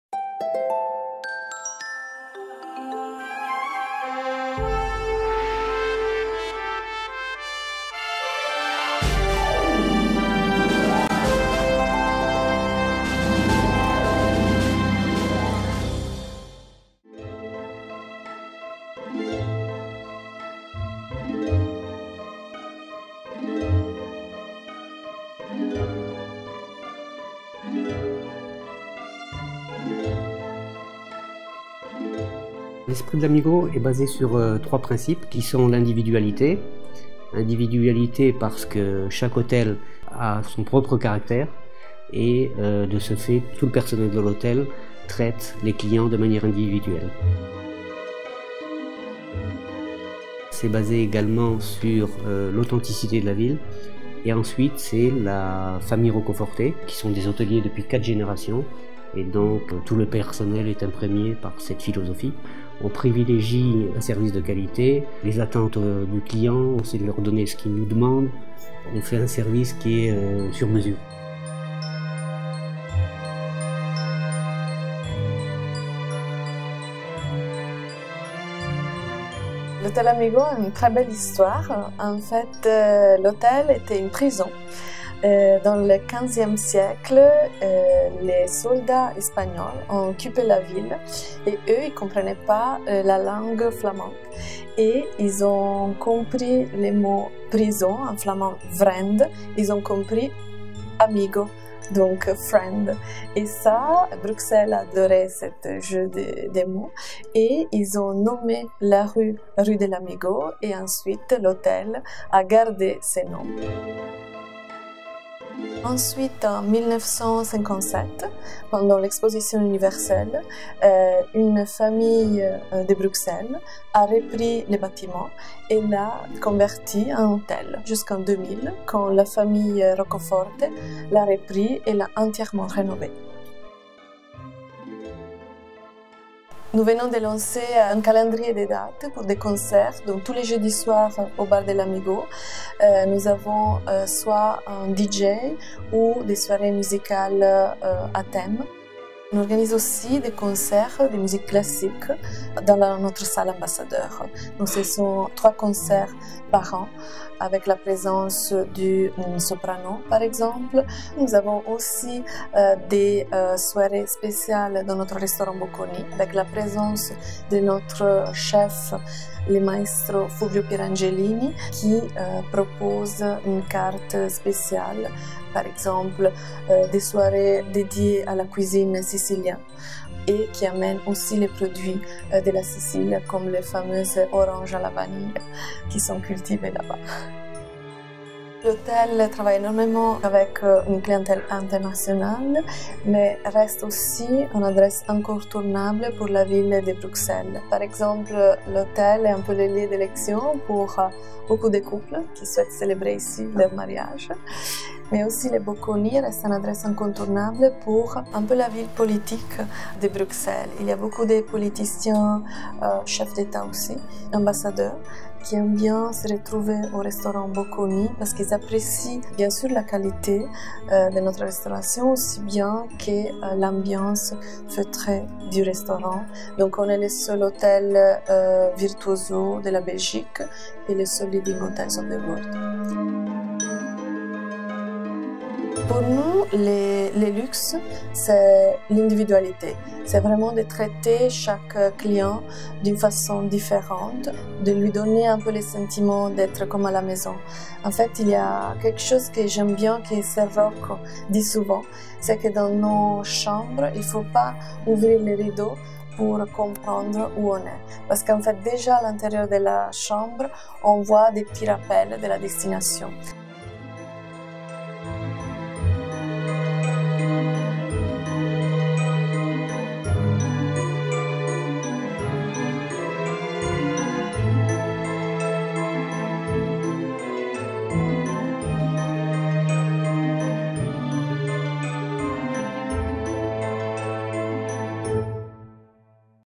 Face à face avec Silvia Brighenti et Philippe Spagnol - Hôtel Amigo Bruxelles
La promesse de moments mémorables
C’est au cœur du centre historique de Bruxelles, à quelques encablures de la Grand Place, que se niche l’un des plus beaux hôtels de la ville. Seul hôtel de Belgique membre du prestigieux label Leading Hotels of the World, l’hôtel Amigo allie avec intelligence raffinement, élégance et confort ultra moderne.